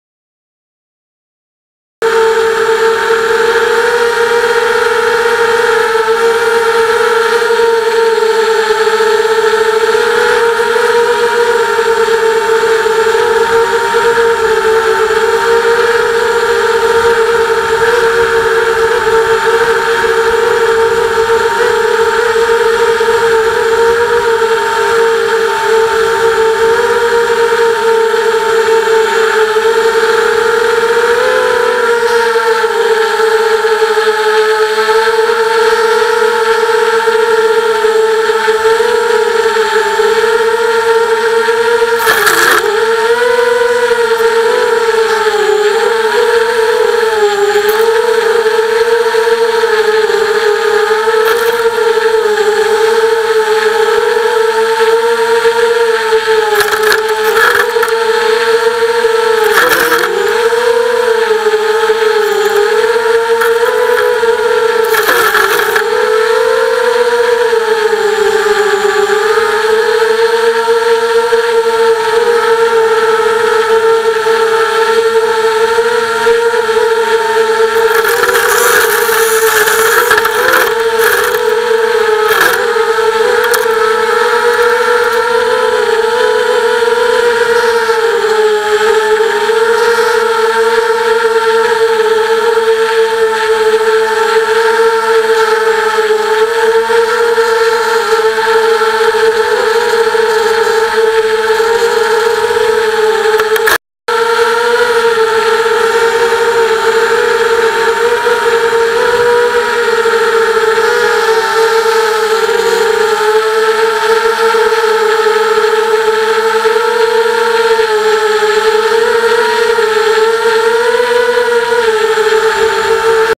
Tree inspection and leaf sampling with a drone
Aim was to see if I could get my micro sized drone to move around within a trees canopy and use its propellers to cut/sample leaves.

Tough eucalyptus leaves proved too tough for the propellers, got a few down but not that successful.  Will try a few cutting attachments next.

Even given the wind it was possible to control the drone in relatively small spaces and target specific groups of leaves.  The flight controller seemed to cope well with the contact and was able to compensate.

Drone weighed less than 250g, runs a Naze32 flight controller running Betaflight with Horizon mode on for stability.  Propeller  tip to tip measures 230mm.

Propellers are stubby ABS plastic, not very efficient but robust enough not to shatter on contact.